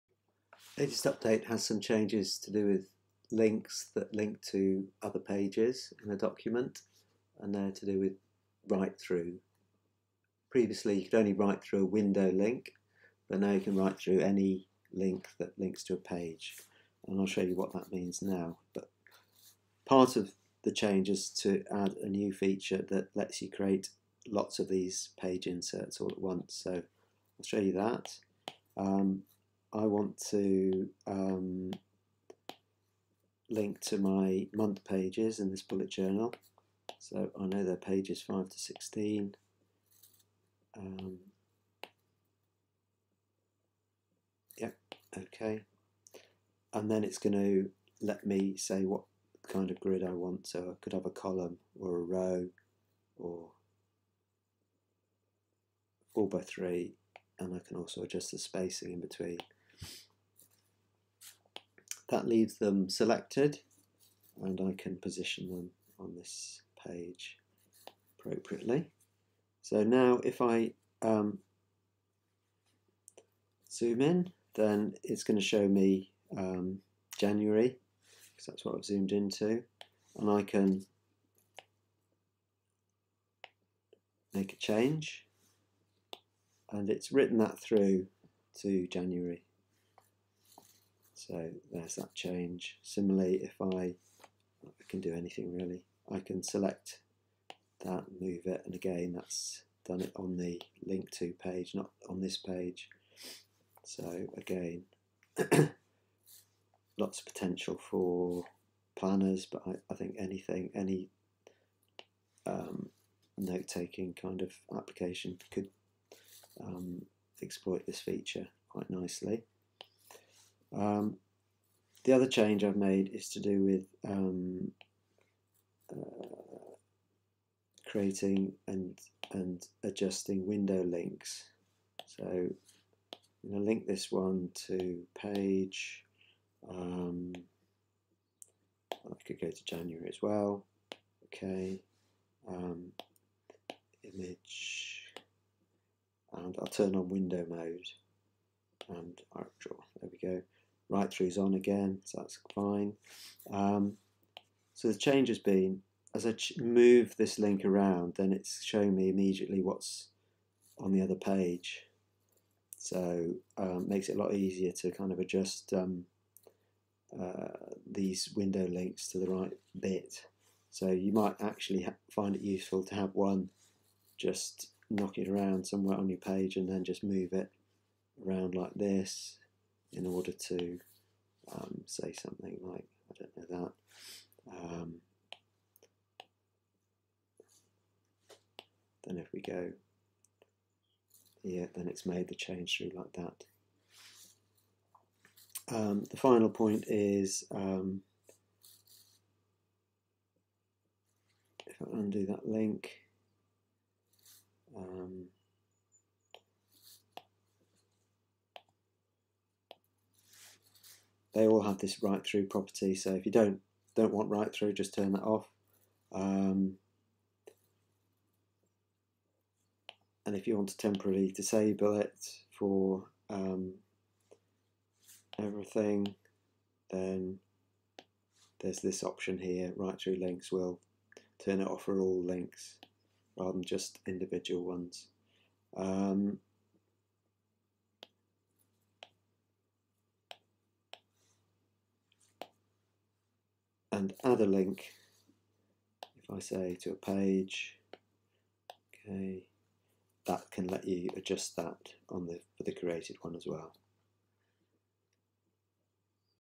Write-through links in ZoomNotes
You can now turn on 'Write-through' on any links (to other pages in a document) in ZoomNotes. This lets you edit the linked-to pages directly from the page containing the link.